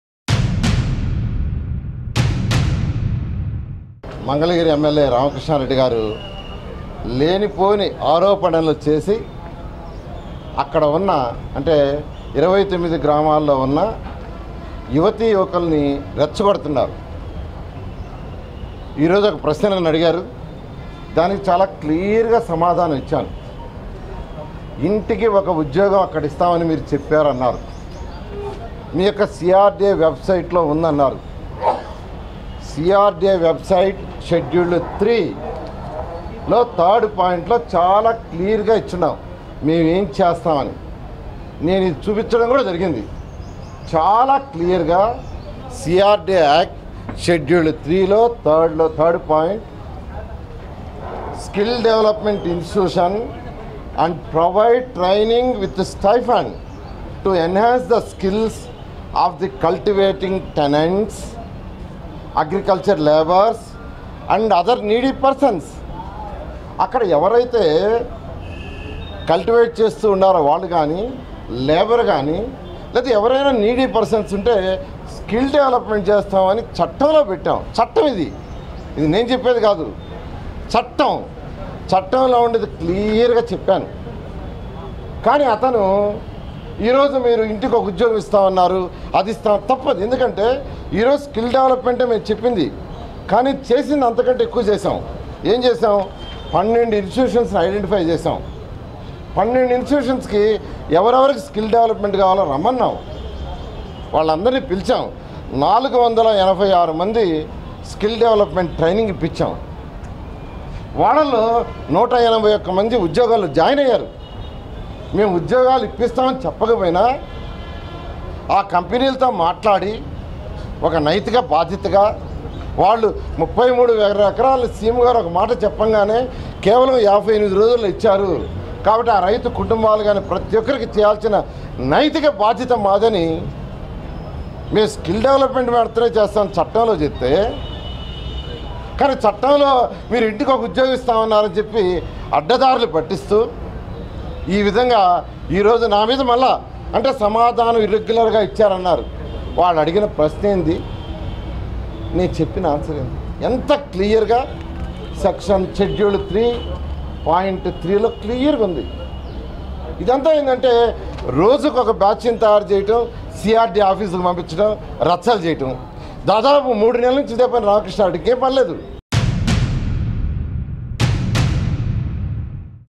TDP MINISTER P NARAYANA CLARIFIES COMMITMENT GIVEN TO THOSE GIVEN LANDS FOR CAPITAL
TDP MINISTER P NARAYANA CLARIFIES COMMITMENT GIVEN TO THOSE GIVEN LANDS FOR CAPITAL. Watch Assembly Videos